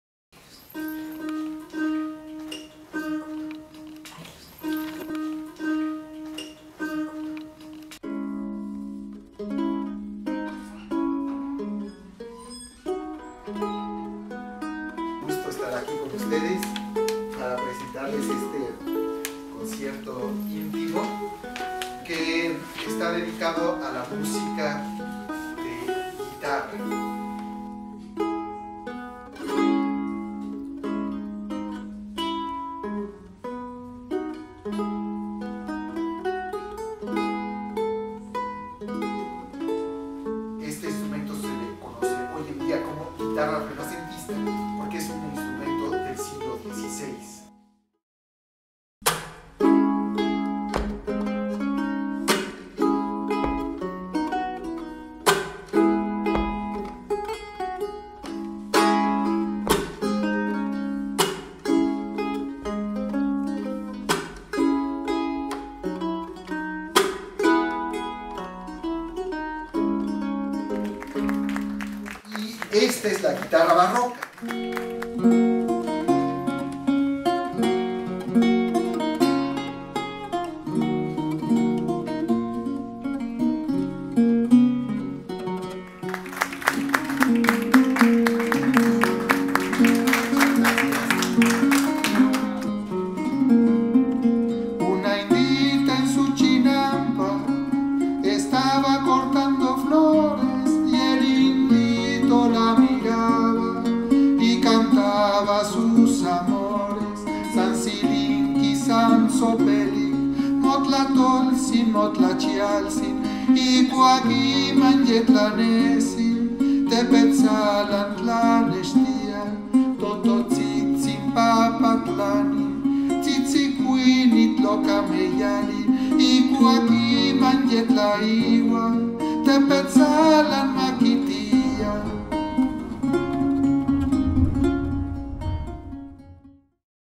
Manuel Mejía Armijo-conciertos en tu casa
Concierto de guitarras antiguas: guitarra renacentista y barroca
Intérprete: Manuel Mejía Armijo
Conciertos en tu casa es una forma de vivir la música en comunidad, tener una experiencia de conocimiento, deleite y convivencia con el catalizador de la música.
El anfitrión convoca a sus invitados a su casa y organiza el calendario, horario y si establece alguna formalidad. El patrocinador se encarga de los honorarios del artista y también convocará invitados en común acuerdo con el anfitrión.
En este caso la patrocinadora fue S. M. Ochoa, la anfitriona fue Estela Ruiz, y en la cámara y edición del vídeo Mariana Idueta.
Coyoacán, México, 2018